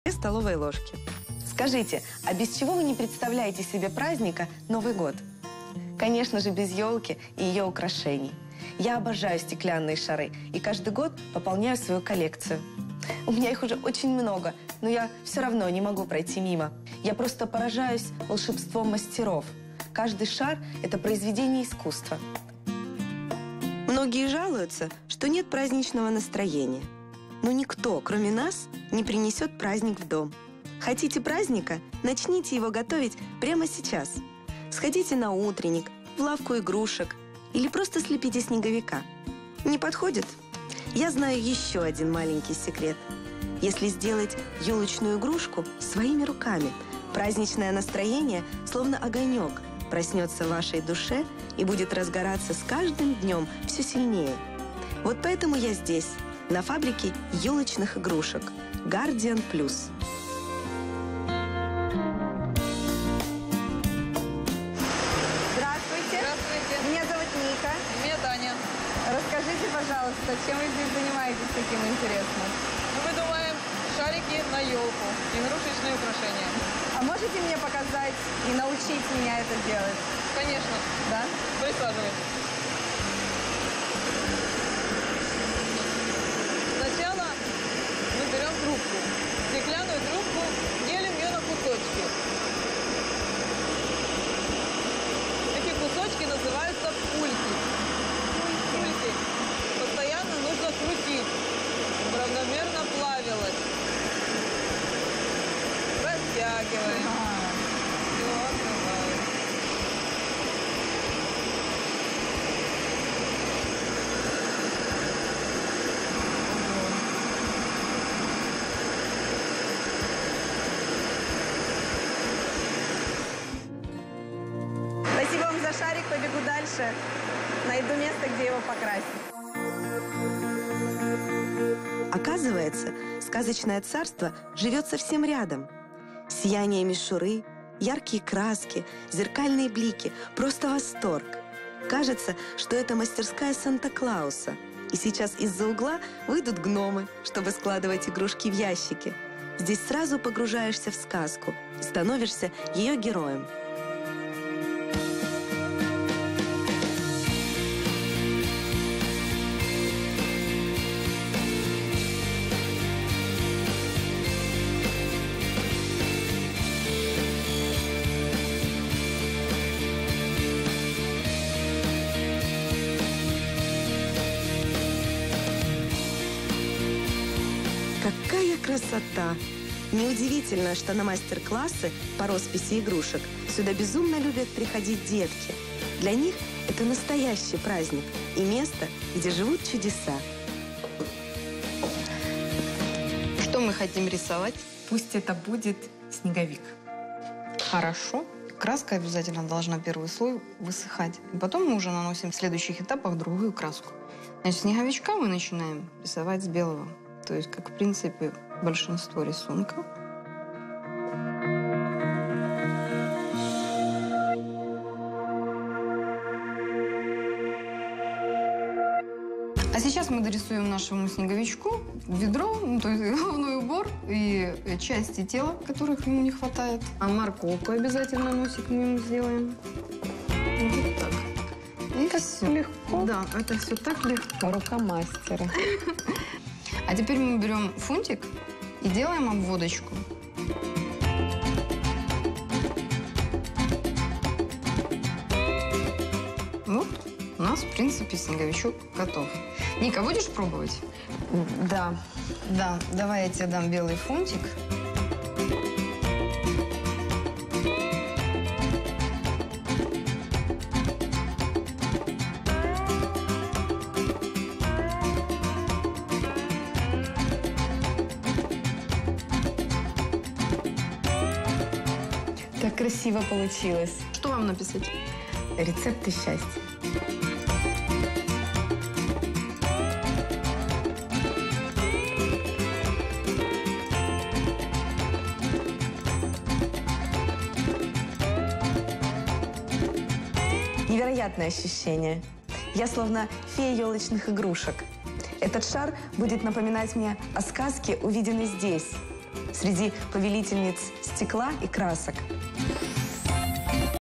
Фабрика елочных игрушек. Ника в гостях.
Елочные игрушки - это обыкновенное чудо! Посмотреть, кто и как это чудо создает, отправилась Ника Досаева.